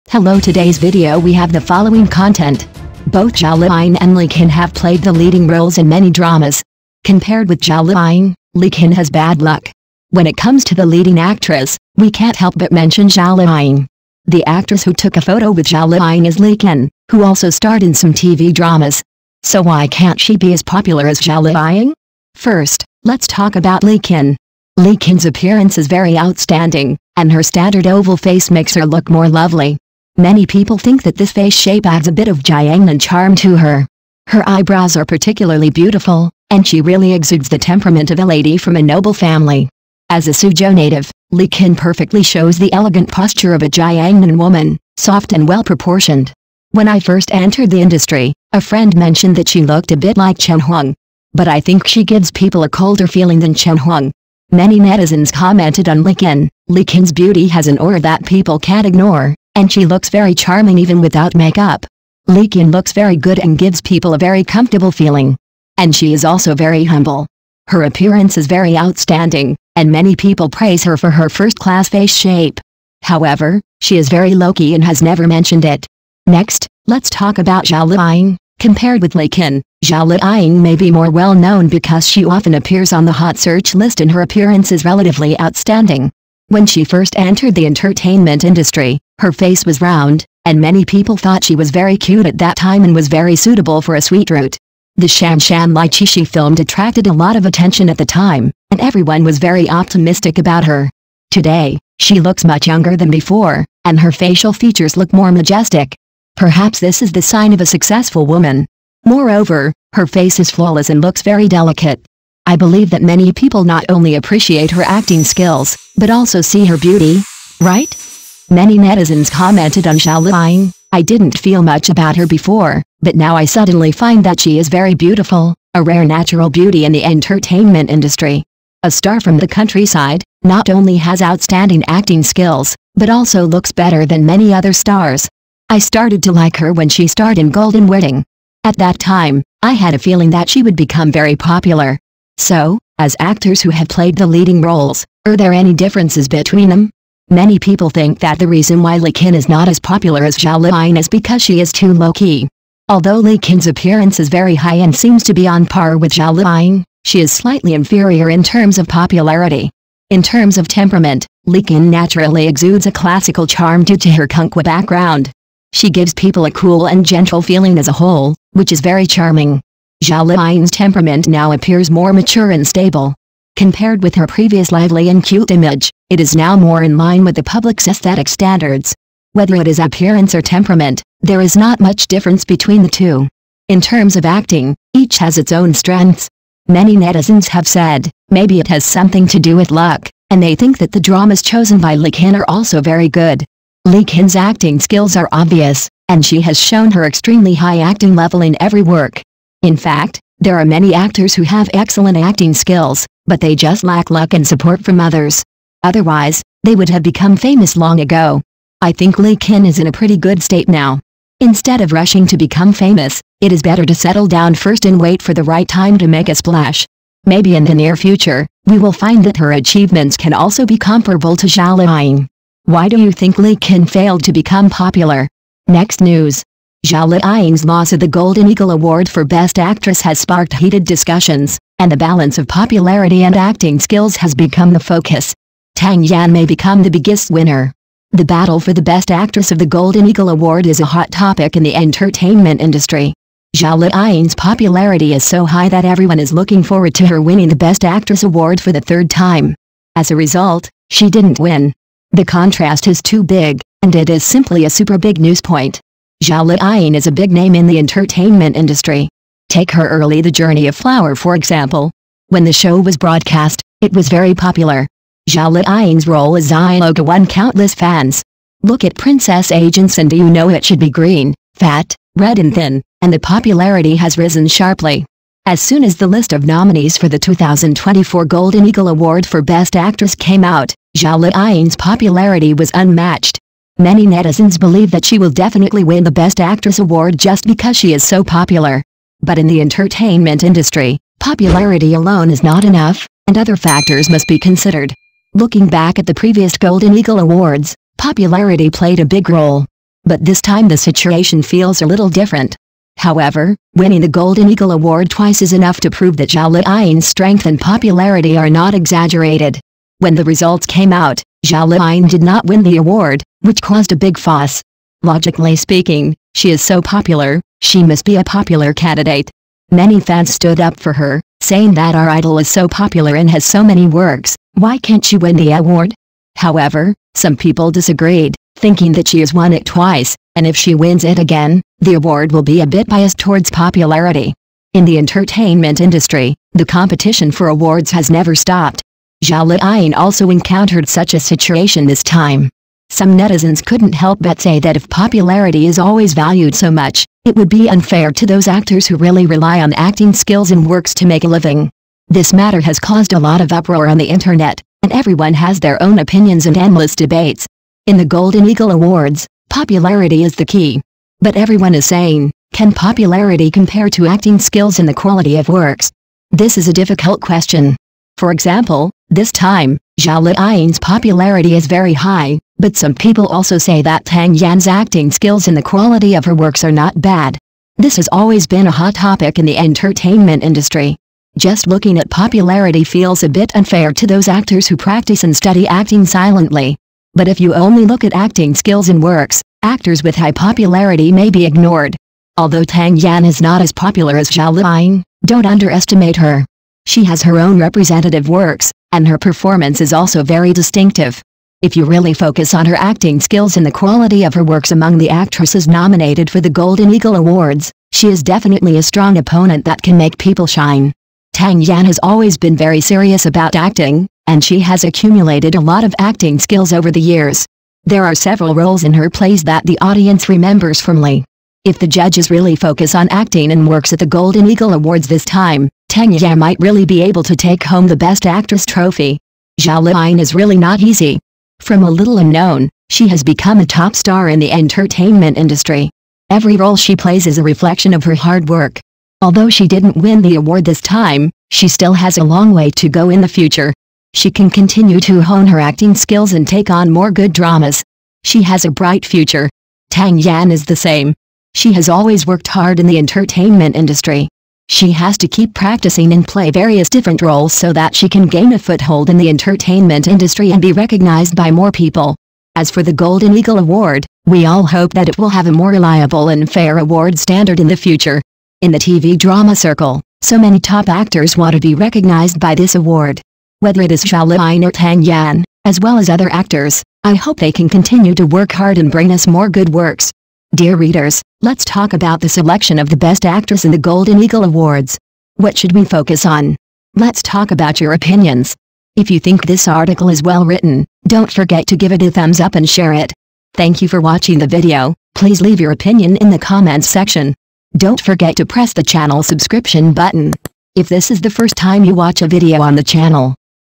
Zhao Liying and Li Qin both played the leading roles in many dramas. Compared
Zhao Liying and Li Qin both played the leading roles in many dramas. Compared with them, Li Qin had bad luck.